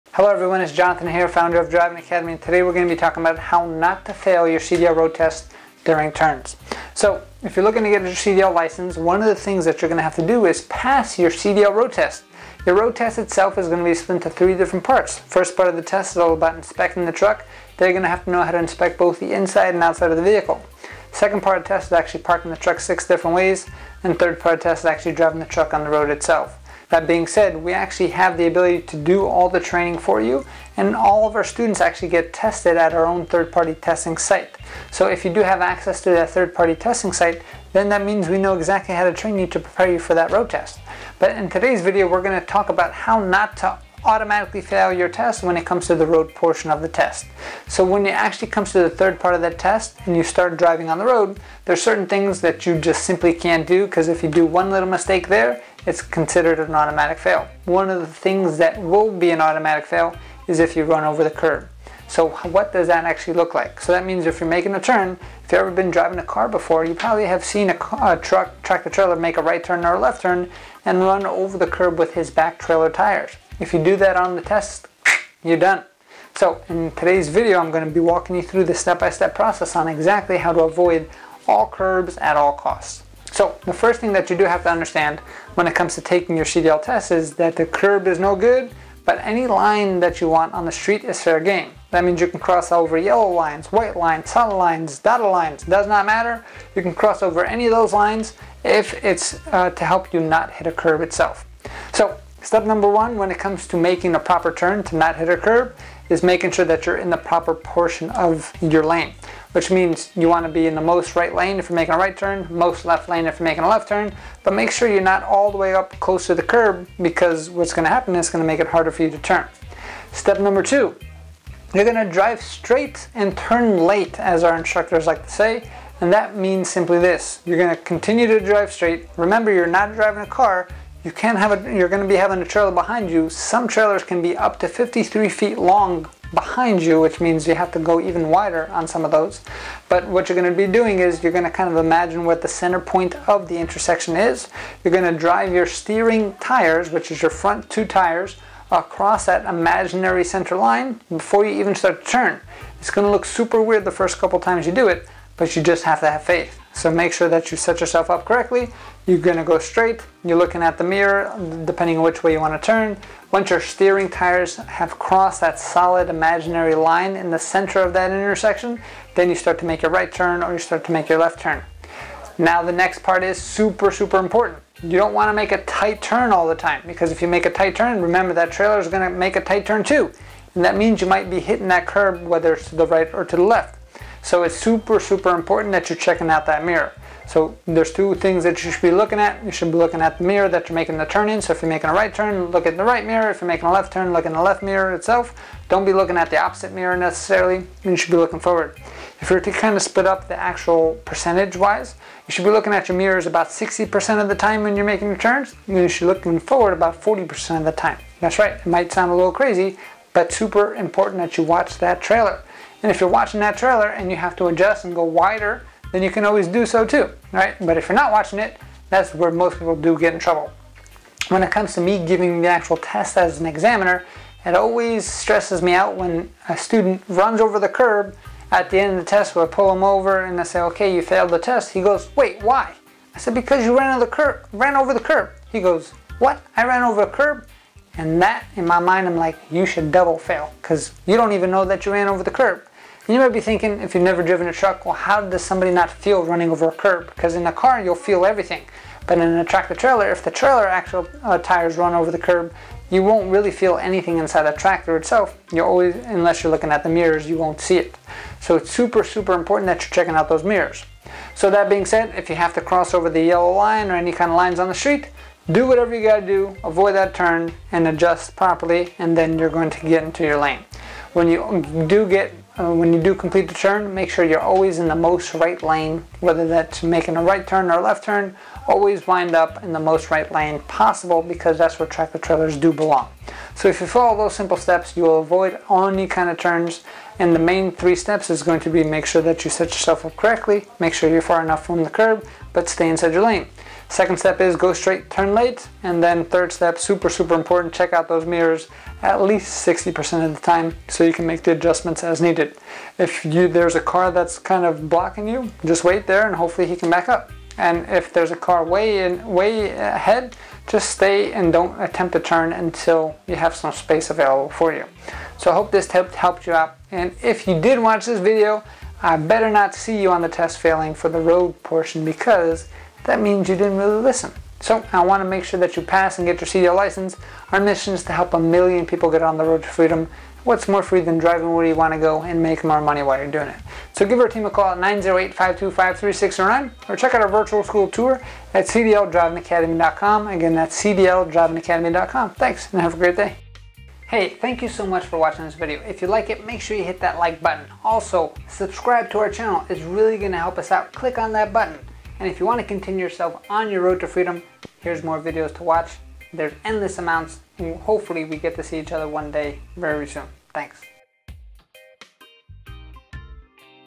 Avoid Failing Your CDL Road Test on TURNING (Avoid Curbs) - CDL Driving Academy
During the CDL road test you will fail if your wheels hit the curb - it is an automatic fail. How can you avoid hitting a curb when turning a huge CDL vehicle? Jonathan from CDL Driving Academy is here to explain how to avoid failing your CDL road test when making a turn!

200 East Edgar Road, Linden NJ